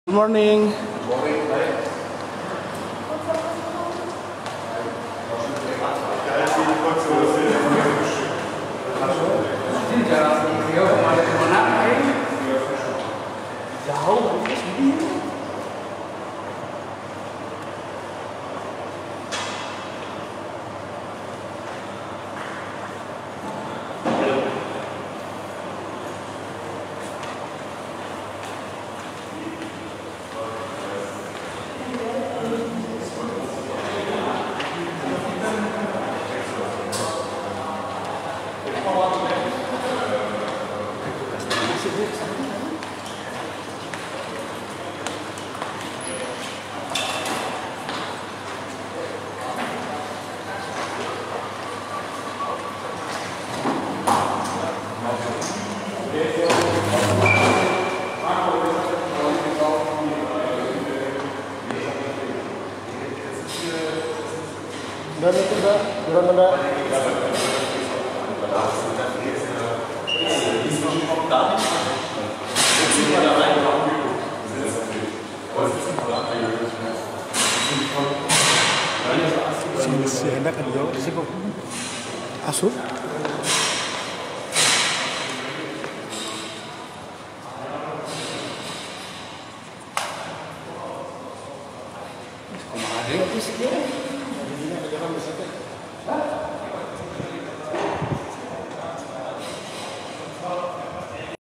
Tim Panser Jerman Gelar Latihan di Stadion Si Jalak Harupat Jelang Laga Grup F Piala Dunia U-17
TRIBUNCIREBON.COM - Penampakan Tim Panser Jerman saat Latihan di Stadion Si Jalak Harupat jelang Laga Grup F Piala Dunia U17, Selasa (7/11/2023).

Reporter: Cipta Permana